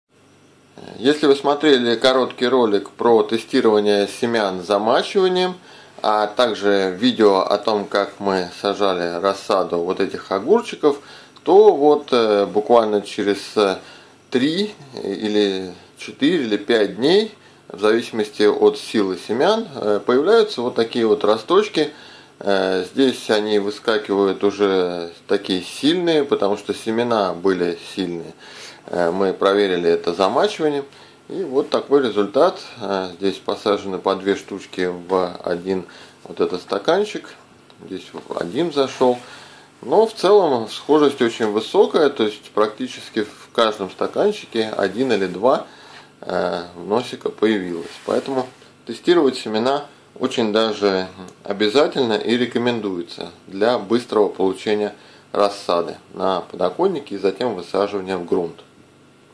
ahoot.ru ОГУРЦЫ САДИМ ТРИЖДЫ в СЕЗОН
ahoot.ru - каждый найдёт страничку по вкусу...
Несколько лет назад был найден интересный опыт высаживания рассада огурцов несколько раз за сезон, это удобно для ухода, это распределяет количество получаемого урожая плодов равномерно и нет необходимости выбрасывать или раздавать излишки. Первыми высаживаются супер ранние сорта для быстрой выгонки раннего сбора плодов огурца, а затем среднеспелые для плодоношения всё лето и даже сильно в осень если теплица хорошая и держит тепло.
Получить рассаду через отбор замачиванием можно максимум за неделю, после чего она готова к высаживанию в грунт.